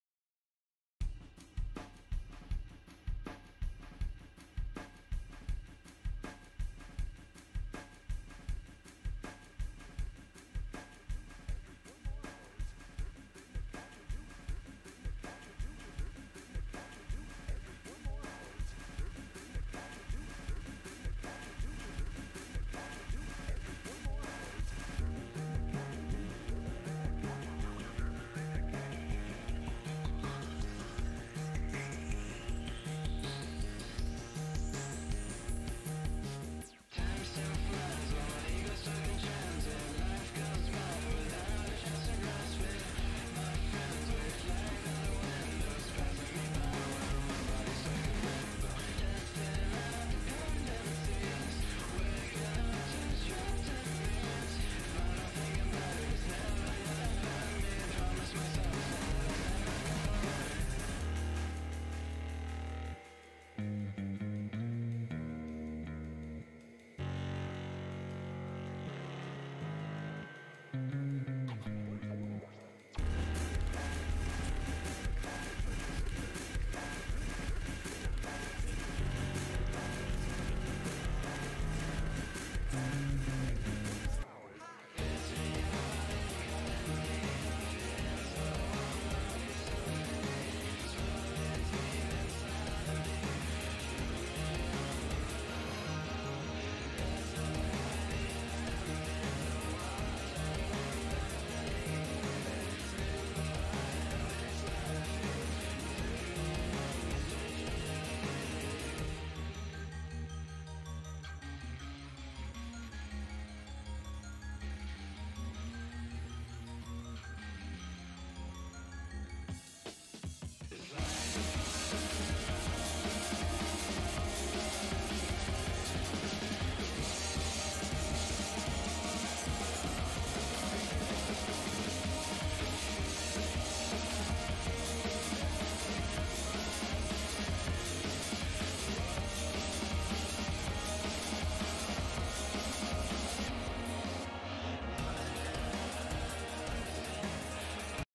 perks preview
it might look like it's lagging behind but trust me that's just from recording while running on battery power

the metronome and clap are entirely in-sync and the waveform is also in-line with the beat markers

lessthan3